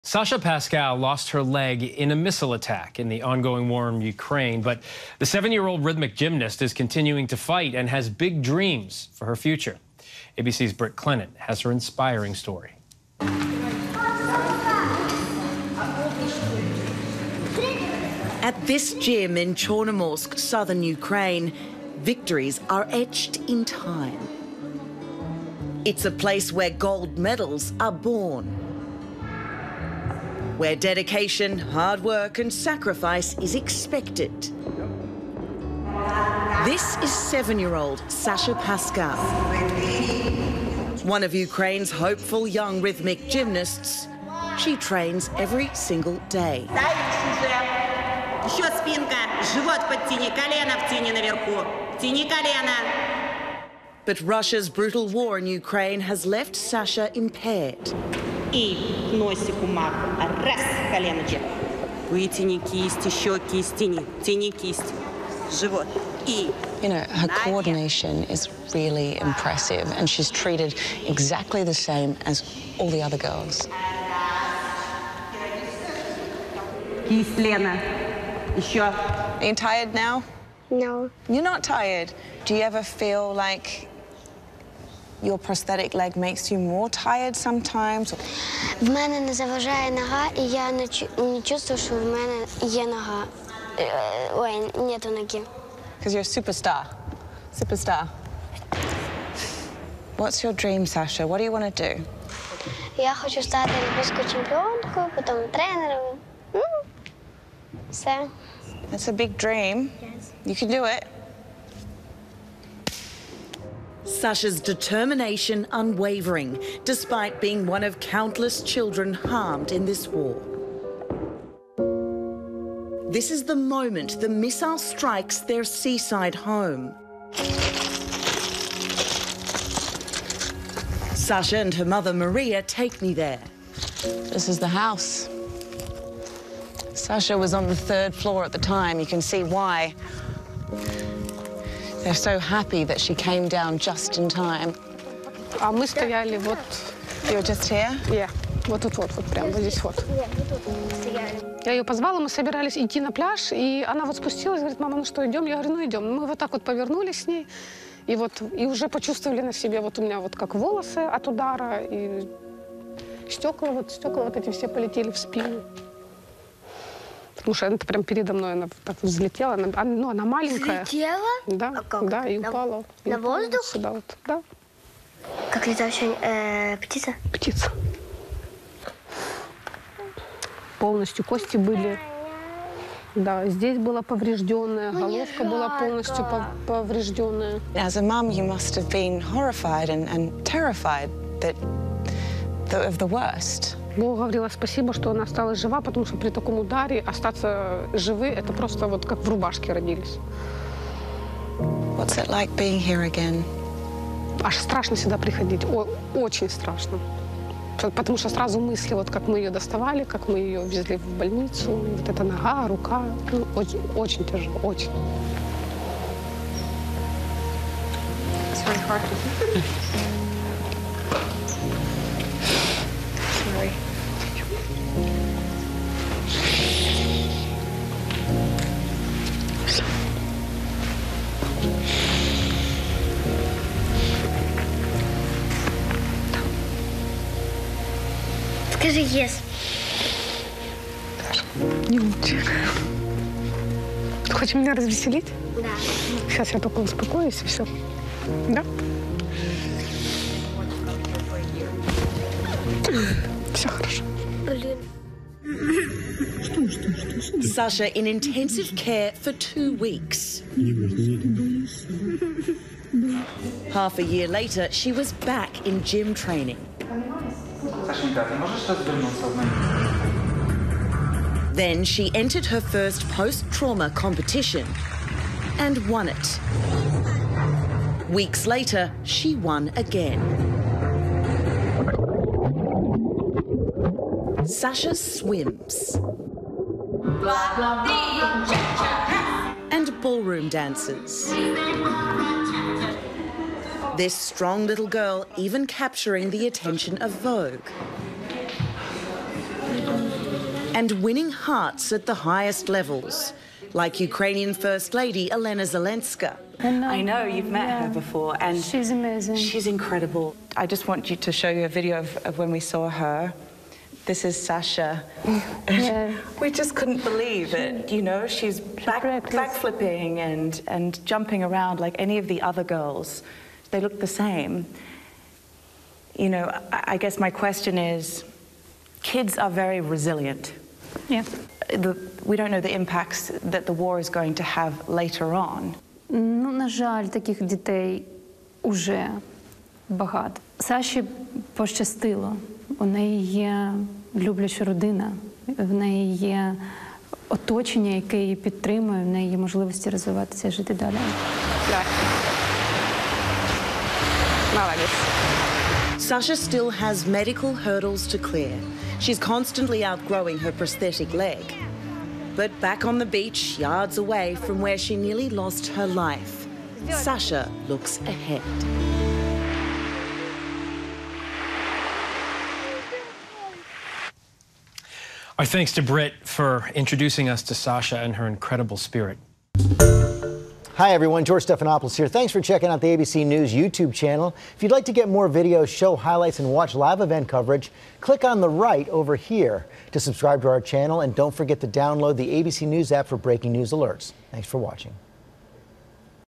Seven-year-old Ukrainian gymnast fights on after losing leg
ABC News' Britt Clennett spoke with 7-year-old Sasha Paskal about how she returned to rhythmic gymnastics after losing her leg in a missile strike, and the big dreams she has for her future.